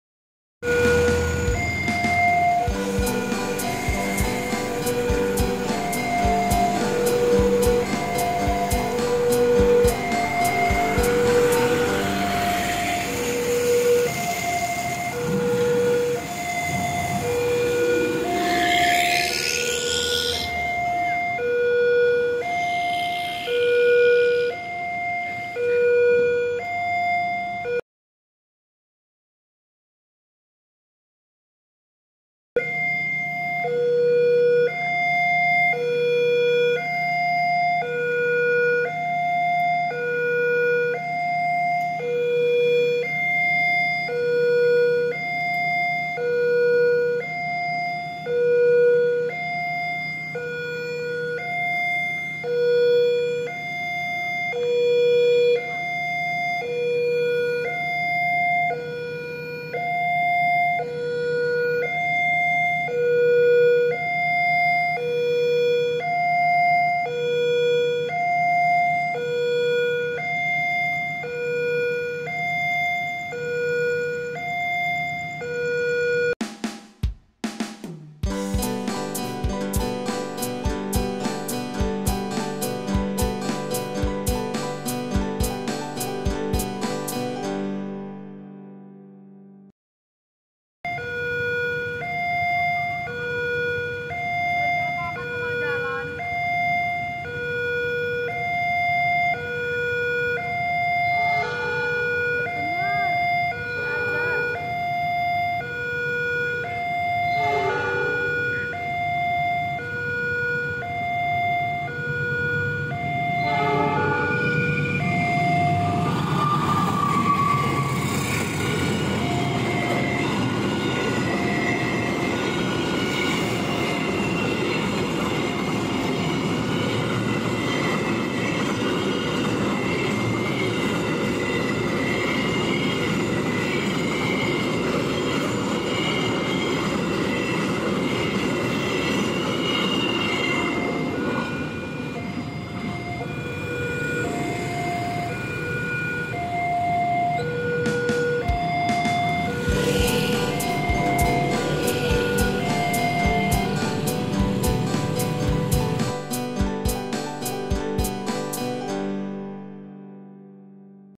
Giliran Limas Priuk Cargo yang Melintas Setelah Argo Bromo Anggrek Malam @KH.Izqil_Channel
setelah KA Argo Bromo Anggrek Malam (ABA) Livery Baru Warna Pink melintas di JPL Ngrombo, dari arah berlawanan juga melintas ngebut KA Limas Priuk Cargo (LPC)